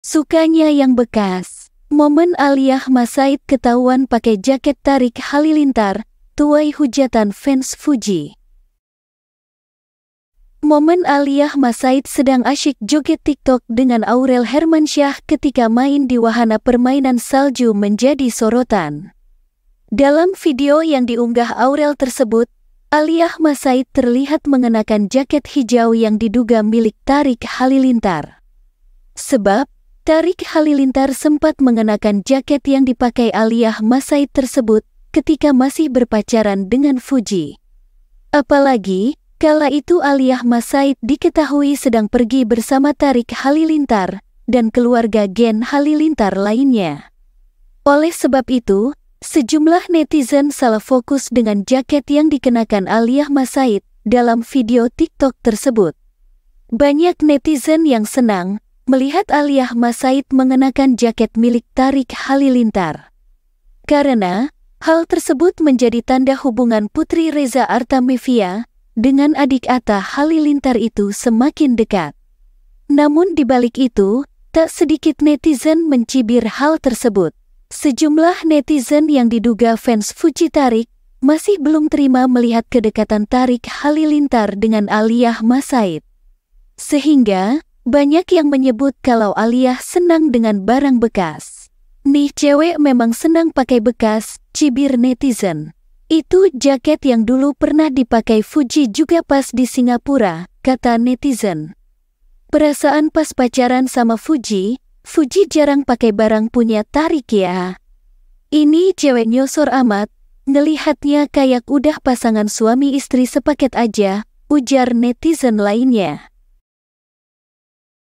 "Sukanya Bekas" Momen Aaliyah Massaid Kepergok Pakai Jaket Thariq Halilintar, Tuai Hujatan Fans Fuji
Momen Aaliyah Massaid sedang asyik joget TikTok dengan Aurel Hermansyah ketika main di wahana permainan salju  menjadi sorotan. Dalam video yang diunggah Aurel tersebut, Aaliyah Massaid terlihat mengenakan jaket hijau yang diduga milik Thariq Halilintar.
Sebab, Thariq Halilintar sempat mengenakan jaket yang dipakai Aaliyah Massaid tersebut, ketika masih berpacaran dengan Fuji. Apalagi, kala itu Aaliyah Massaid diketahui sedang pergi bersama Thariq Halilintar, dan keluarga Gen Halilintar lainnya. Oleh sebab itu, sejumlah netizen salah fokus dengan jaket yang dikenakan Aaliyah Massaid, dalam video TikTok tersebut.
Banyak netizen yang senang, melihat Aaliyah Massaid mengenakan jaket milik Thariq Halilintar. Karena, hal tersebut menjadi tanda hubungan putri Reza Artamevia, dengan adik Atta Halilintar itu semakin dekat.
Namun di balik itu, tak sedikit netizen mencibir hal tersebut. Sejumlah netizen yang diduga fans Fuji-Thariq, masih belum terima melihat kedekatan Thariq Halilintar dengan Aaliyah Massaid. Sehingga, banyak yang menyebut kalau Aaliyah senang dengan barang bekas.
"Nih cewek memang senang pakai bekas," cibir netizen. "Itu jaket yang dulu pernah dipakai Fuji juga pas di Singapura," kata netizen.
"Perasaan pas pacaran sama Fuji, Fuji jarang pakai barang punya Thariq ya. Ini cewek nyosor amat, ngelihatnya kayak udah pasangan suami istri sepaket aja," ujar netizen lainnya.